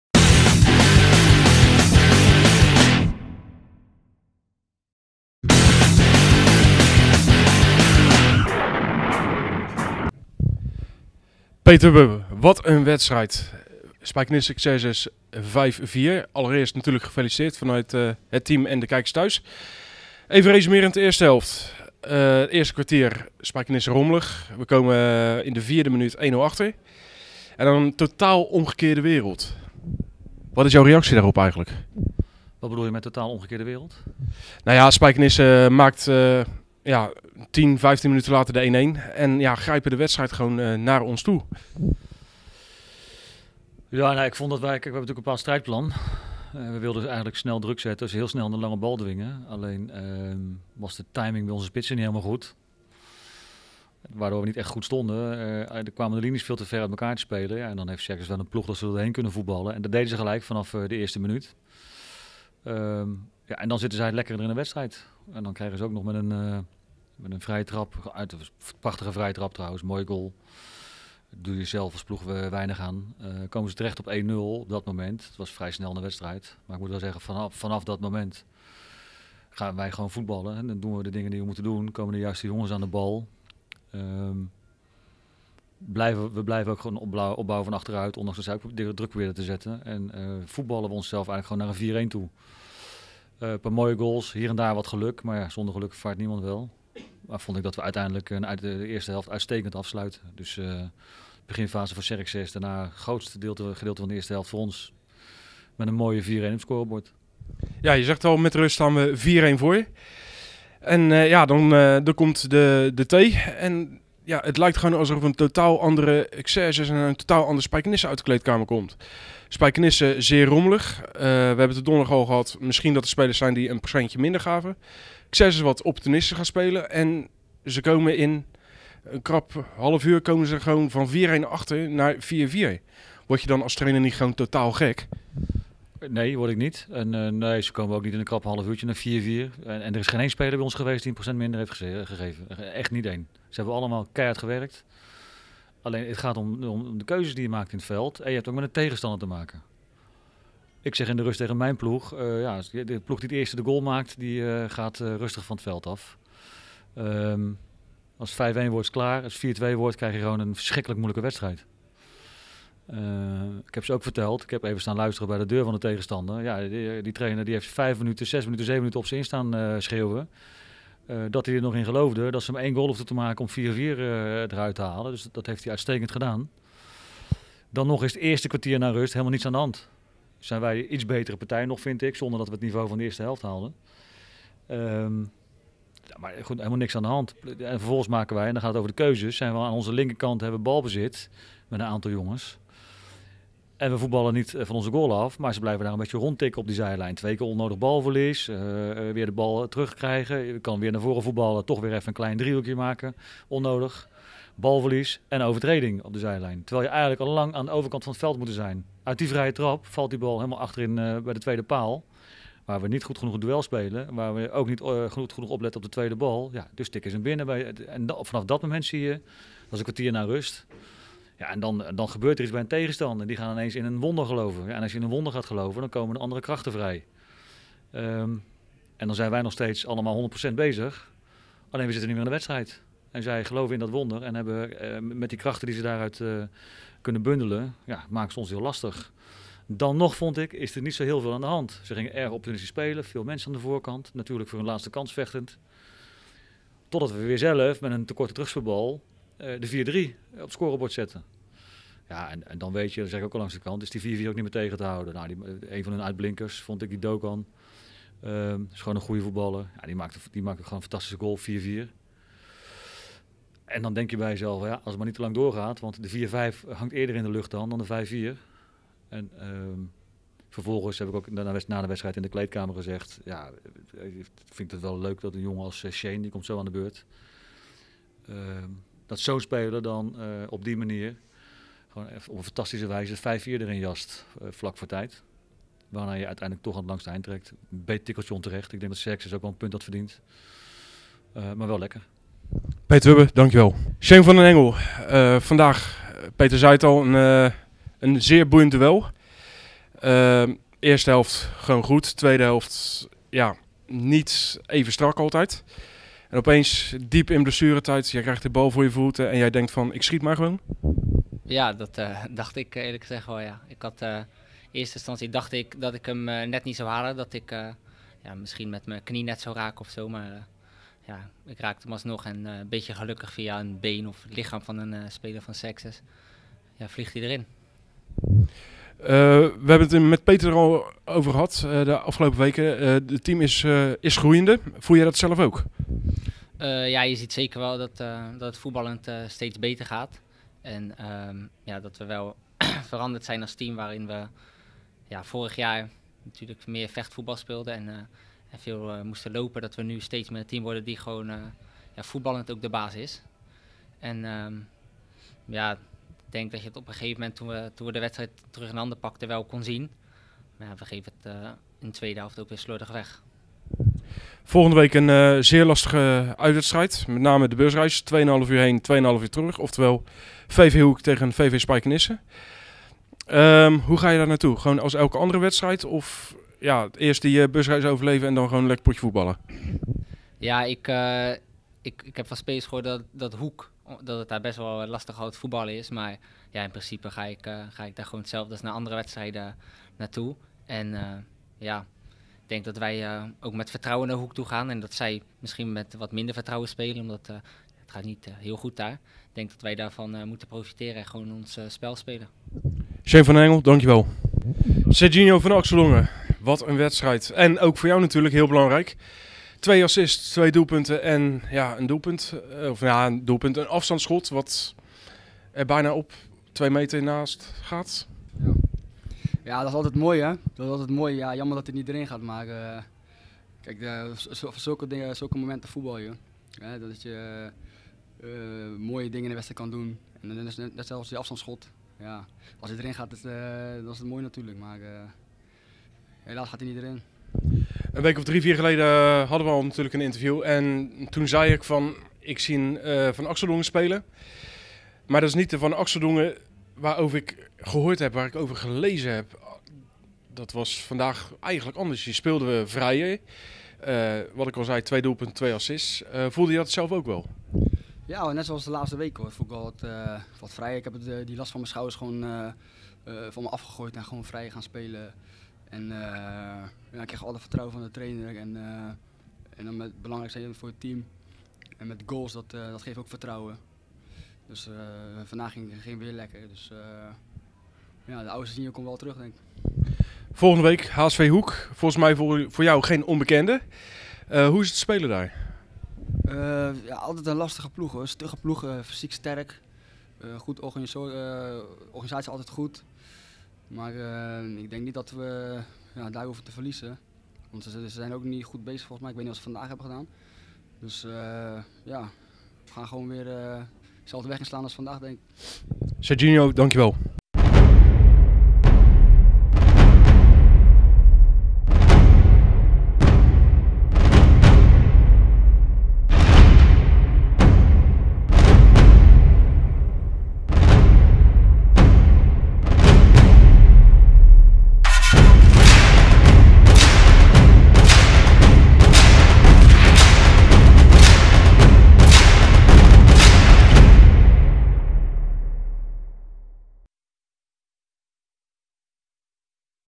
Nabeschouwing Spijkenisse - Xerxes DZB
Nabeschouwing Spijkenisse - Xerxes DZB met Trainer Peter Wubben, Shane van den Engel en Serginho van Axeldongen